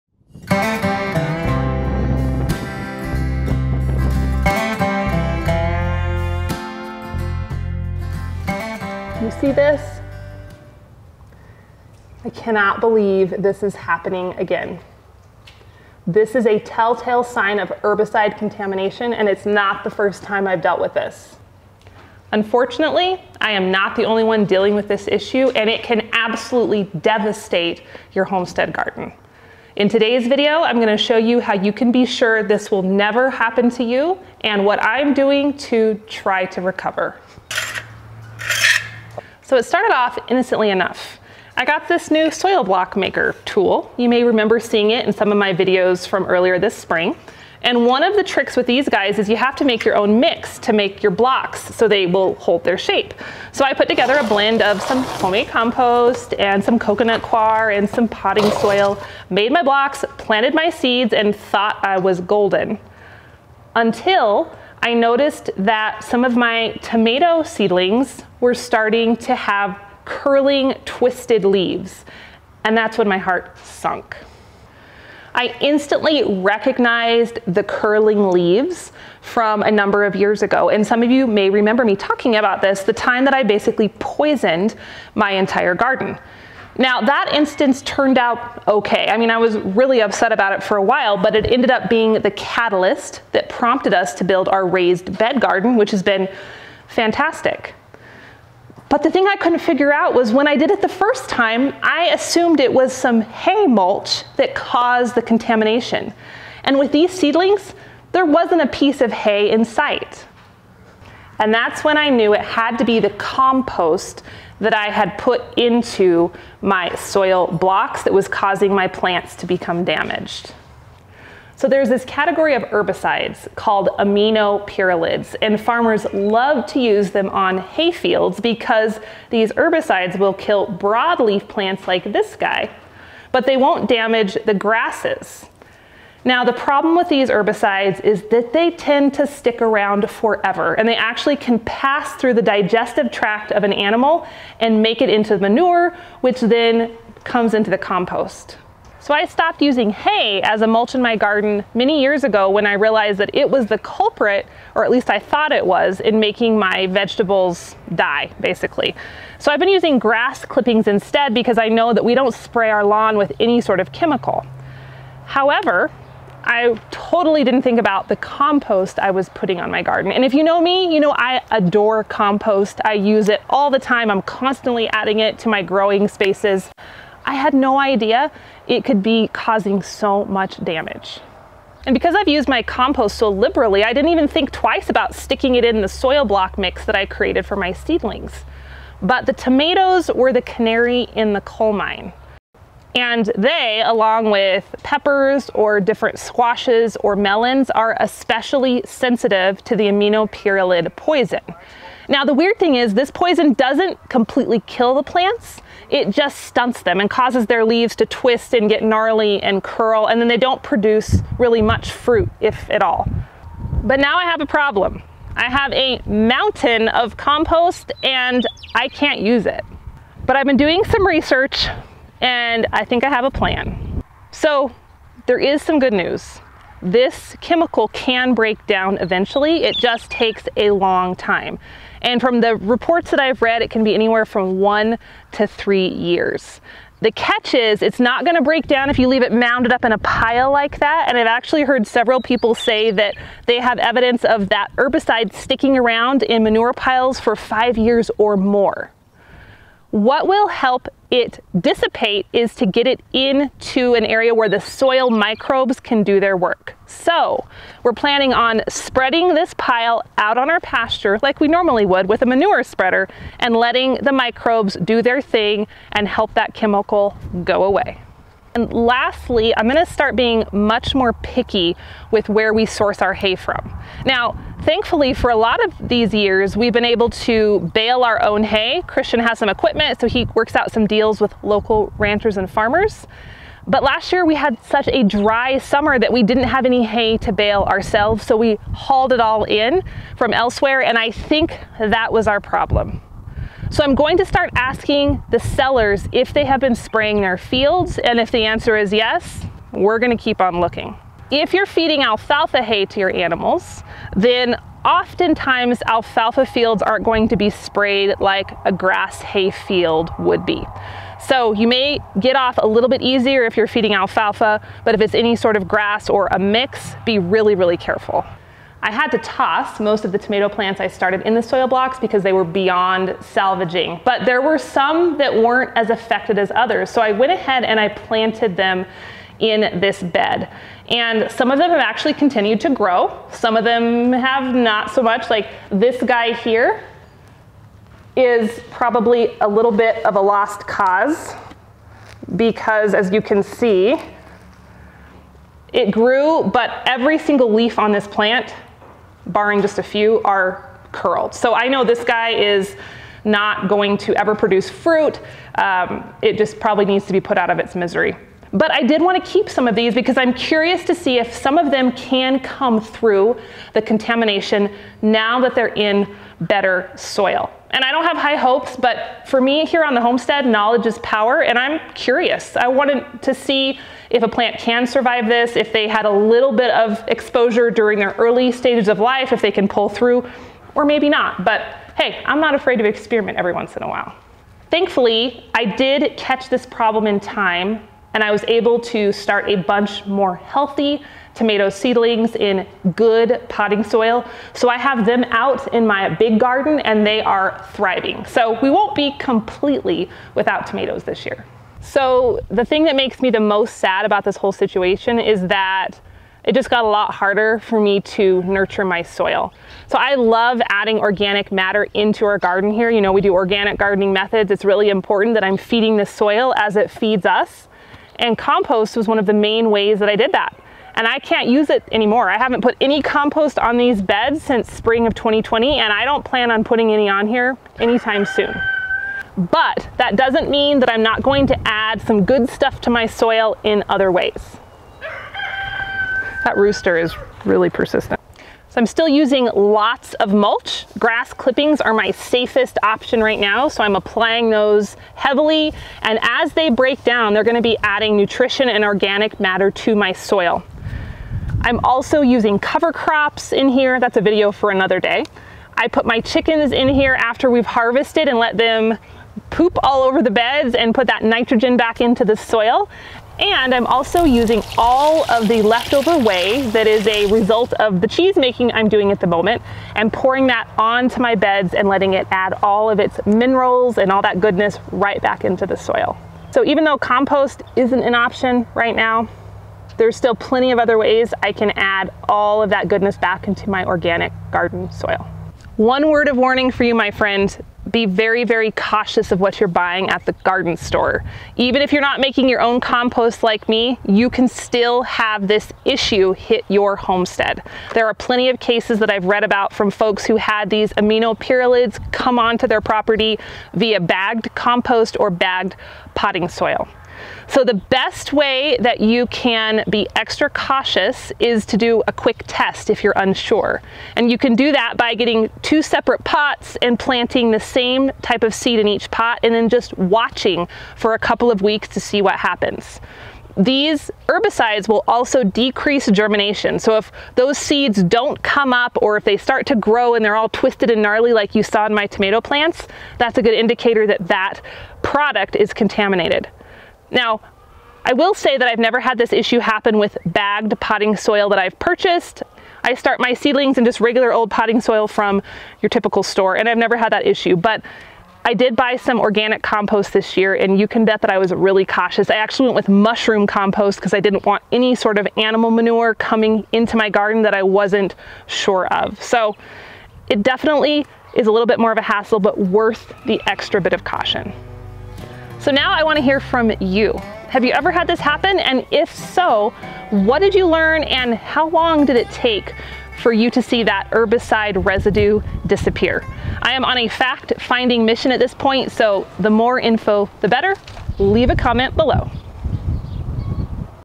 Your Compost May Be KILLING Your Garden…Here's How to Fix It
I couldn't believe my eyes when I saw the telltale curling of my young tomato plants... Turns out the compost I *thought* was nurturing my soil is actually damaging my vegetable plants. Here's the scoop on a phenomenon that's way more common than you think AND how you can combat it!

The Story of How I Poisoned my Garden the First Time

🍅 Create a More Efficient Homestead & Get Mentoring from Jill: freedomfoundry.us